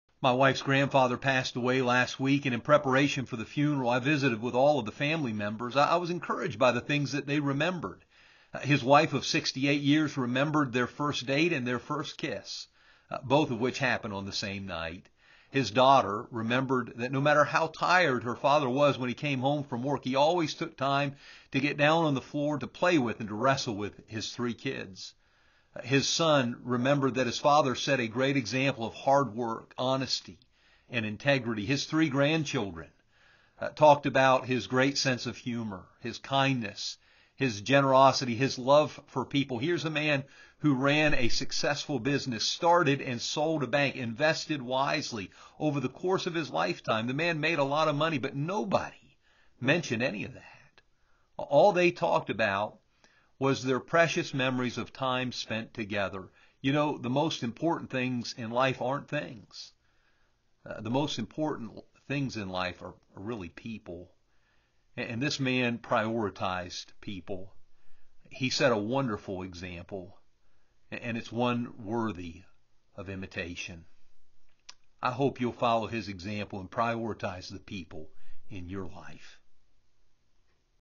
Prioritizing People
My wife’s grandfather, L.A. Spurlin, passed away last week. What people remembered most about him reminded me of something very important. I hope you will imitate his good example.  Prioritize People!